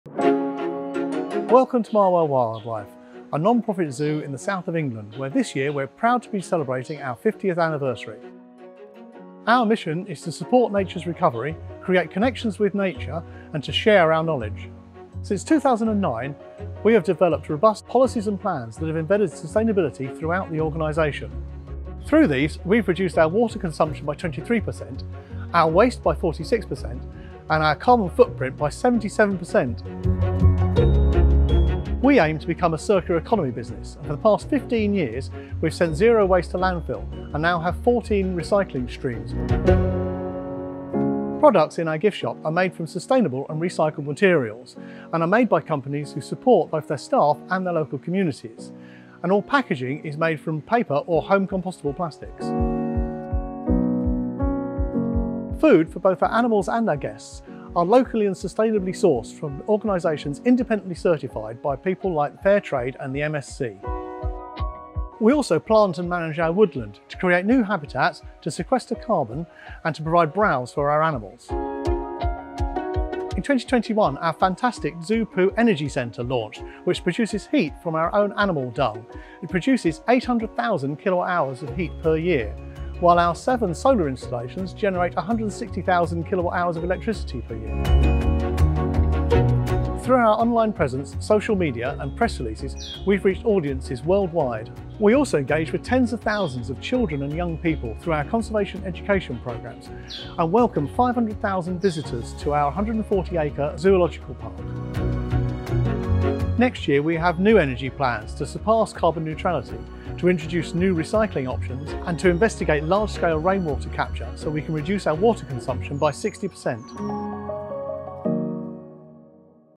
Your visits are helping sustainability! 🌿
Learn about our sustainability initiatives at Marwell Zoo. Your visits help us to complete important projects like these, so thank you for your support!